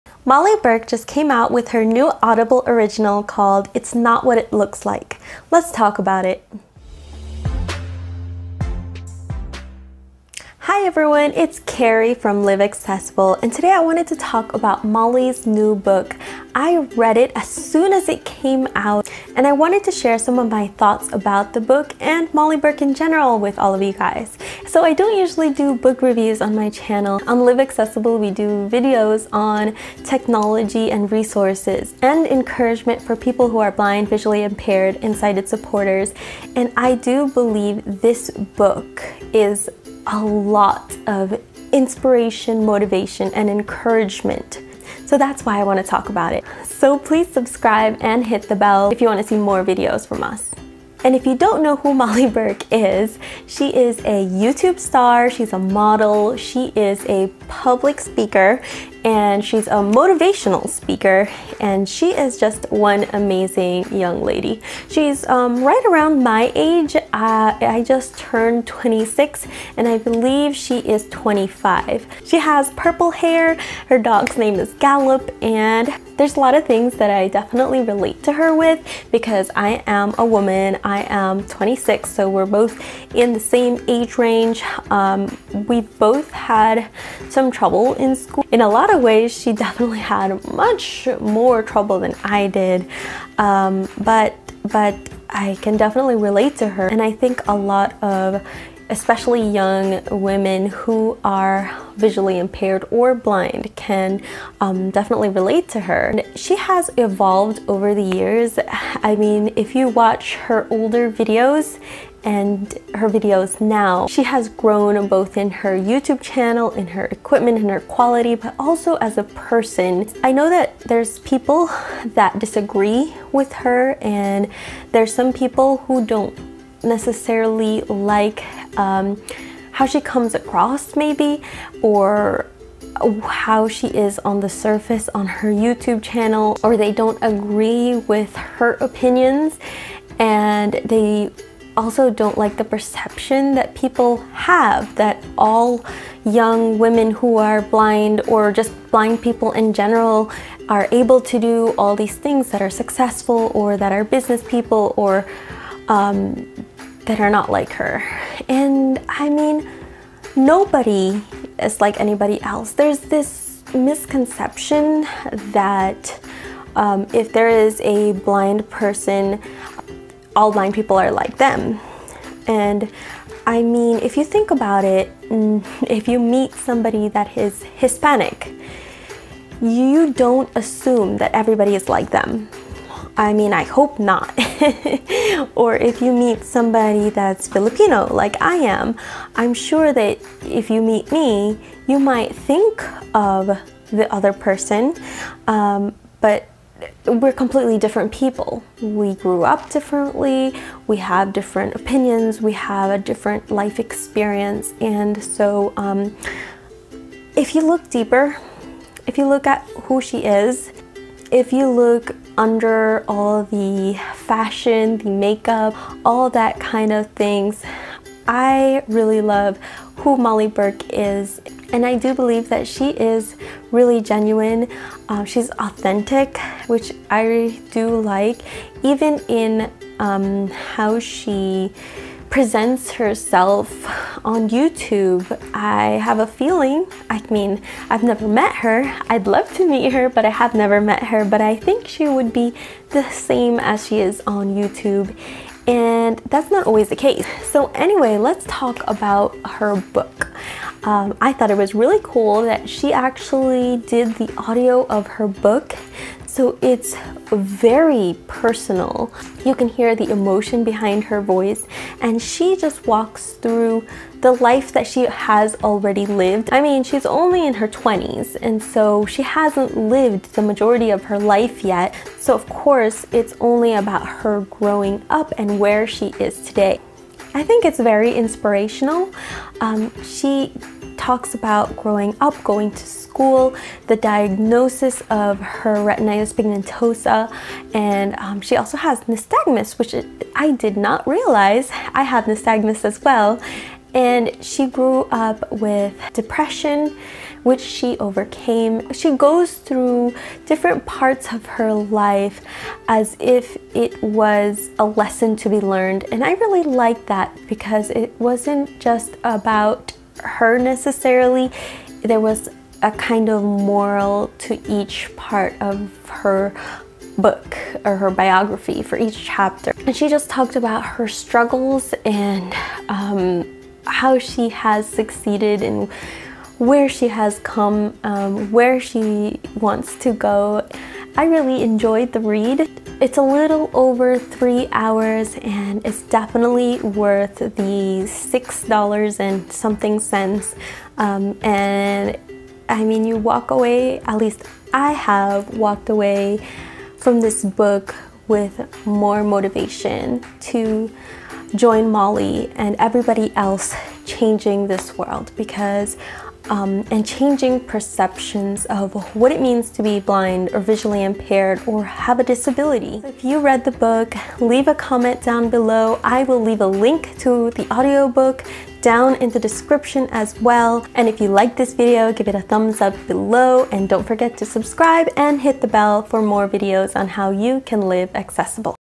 Molly Burke's New Book: "it's not what it looks like" IMPRESSIONS!
___________________ Molly Burke came out with her new audible original book that is an incredible read! I read it the day it came out and wanted to share it with all of you in case you haven't heard of it or you have been on the fence.

"It's not what it looks like" - By Molly Burke